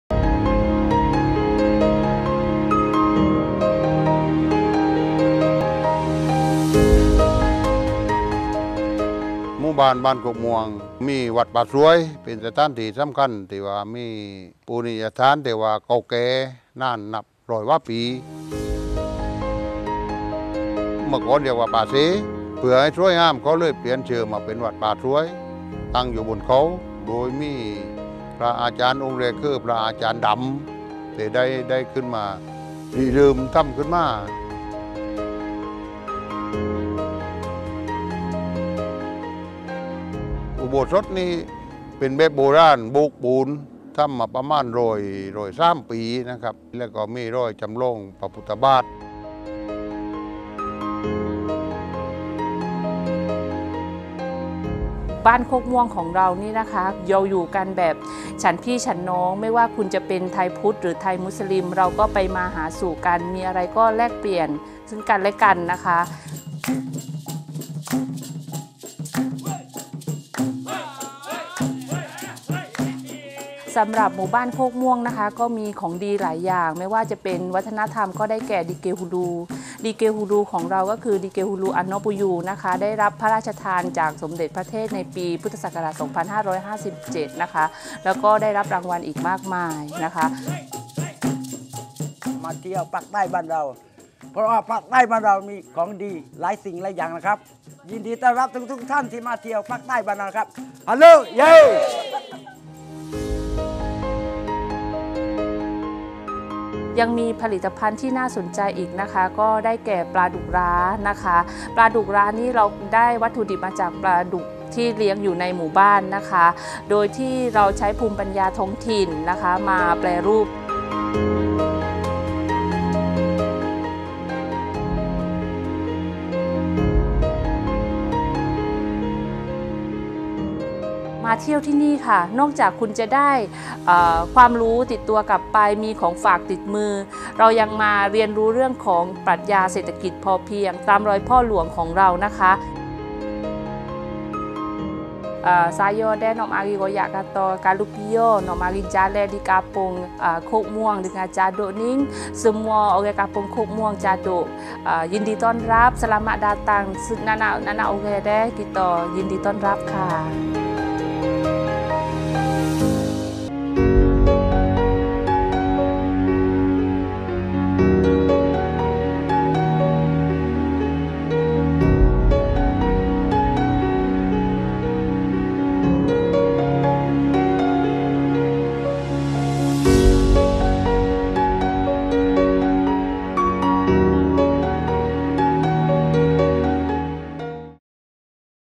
ป่าสวยแม่ลานอยู่ตรงไหน?
บ้านโคกม่วง ต.บนแม่ลาน อ.แม่ลาน จ.ปัตตานี มีวัดป่าสวยเป็นสถานที่สำคัญ และดิเกฮูลูคือสิ่งที่ชาวบ้านที่นี่อนุรักษ์ไว้แก่ลูกหลาน